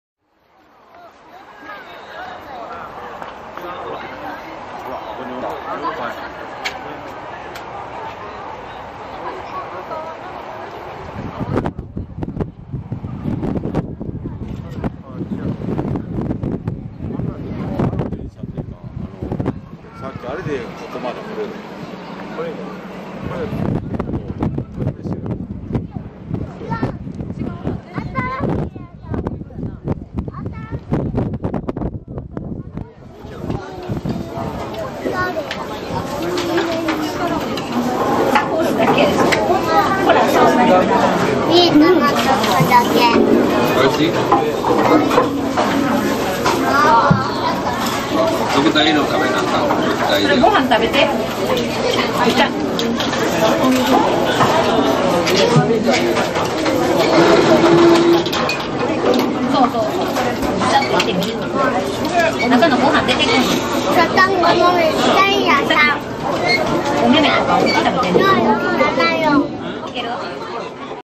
６輌全てが丹後の海（KTR8000形）で編成された特急はしだて＆まいづる
今のところ誰もアップされていないようなので、一応アップロードしておきます。水戸岡デザインの京都丹後鉄道（丹鉄）の「丹後の海（KTR8000形）」が増産（増改装）され４編成になったので、繁忙期編成の６輌すべてが丹後の海で構成された特急はしだて＆まいづるを撮影する機会を得ました。京都鉄道博物館に遊びに行ったので、スカイテラスから見える丹後の海を撮影すると、６輌全てが丹後の海で編成された特急はしだて＆まいづるでした。折り返しの運用も、レストランで食事をしながら撮影しました。